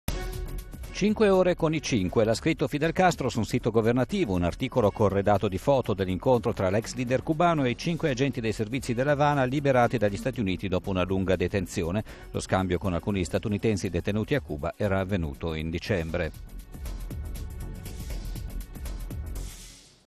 Cuba: Fidel Castro incontra i cinque agenti liberati dagli USA
“Cinque ore con i cinque”: l’ha scritto Fidel Castro su un sito governativo. Un articolo corredato di foto dell’incontro tra l’ex leader cubano e i cinque agenti dei servizi dell’Avana liberati dagli Stati Uniti dopo una lunga detenzione. Lo scambio con alcuni statunitensi detenuti a Cuba era avvenuto in dicembre.